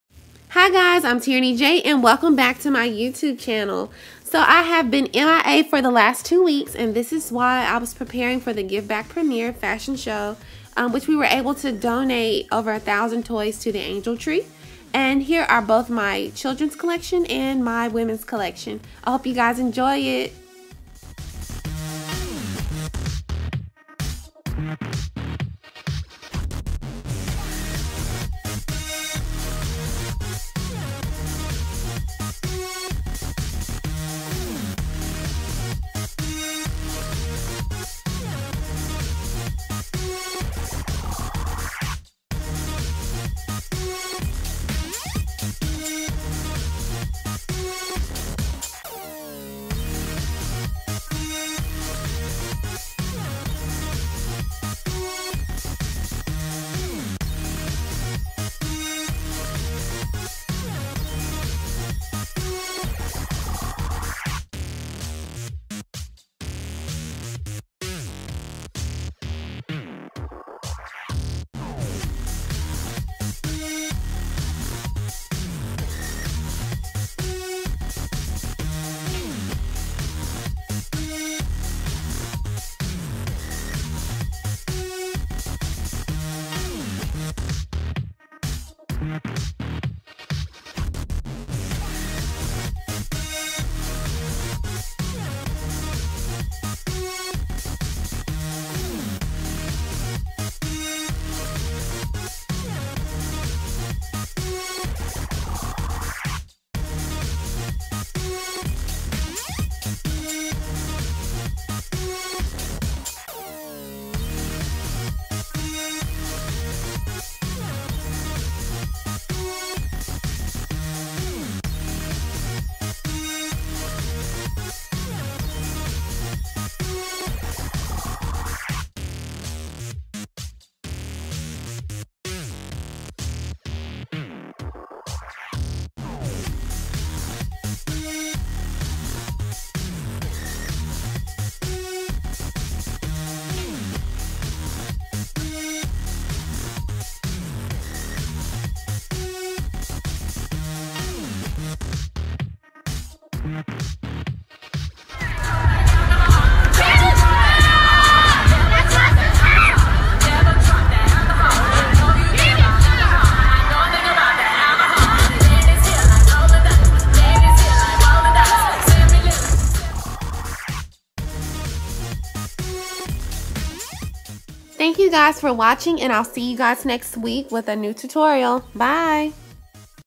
The Giveback Premiere: Fashion Show
Hey loves!!!

So I've been missing in action for a couple of weeks, I had to prepare for my last fashion how of the year! This show benefitted the Angel Tree, bringing over 1,000 toys for children in need during Christmas. I hope you all enjoy my designs. I have two lines, one is called Zippers & Buttons, a fashion kid collection and the other is Teiraneyj, a women's collection.

Teiraneyj.com
Instagram.com/teiraneyj
Twitter.com/teiraneyj
Pinterest.com/teiraneyj